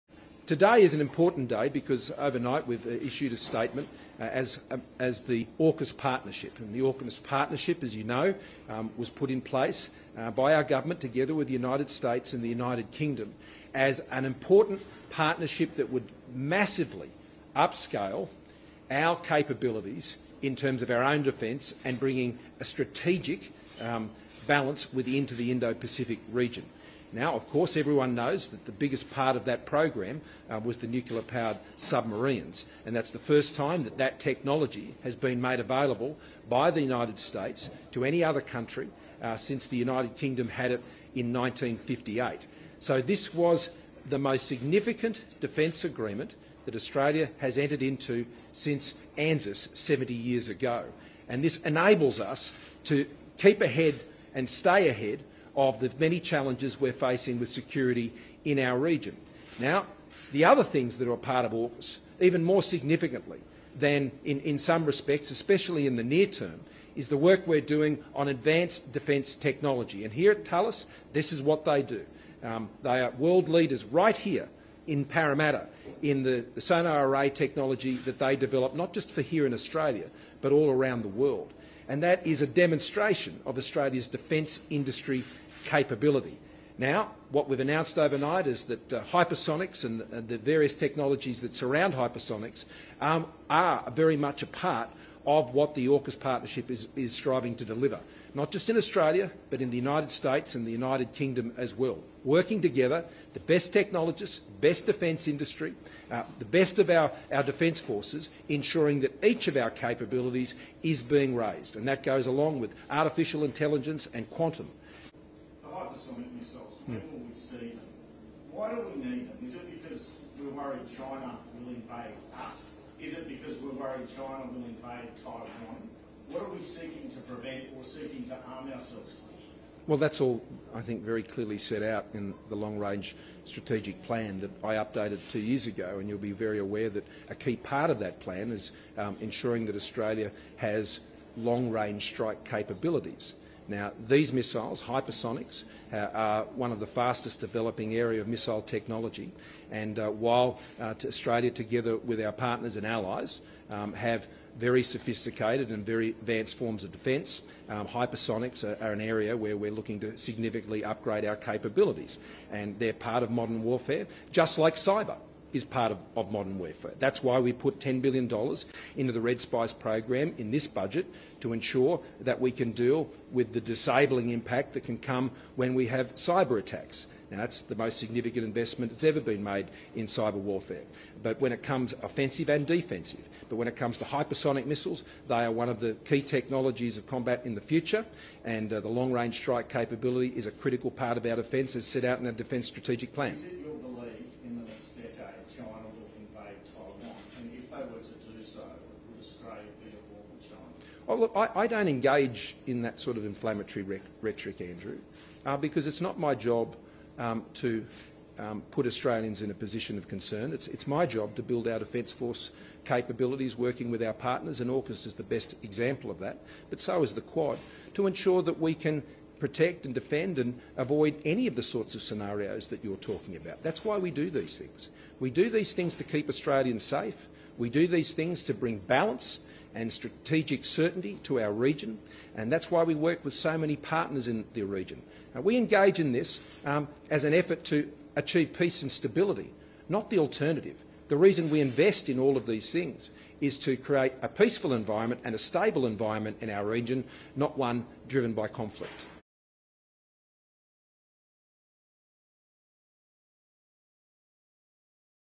AUKUS Working on Hypersonic Missiles to Keep Stability in Indo-Pacific: PM Scott Morrison (4/6/22)
At a press conference in Parramatta, Sydney on Wednesday, Australian Prime Minister Scott Morrison spoke about a new initiative on hypersonic missiles developed by the AUKUS partnership, and its implications for Indo-Pacific stability and Taiwan’s defense amid threats from the People’s Republic of China (PRC). AUKUS is a strategic partnership formed last year between the governments of Australia, the United Kingdom, and the United States which initially provided nuclear-powered submarines to Australia.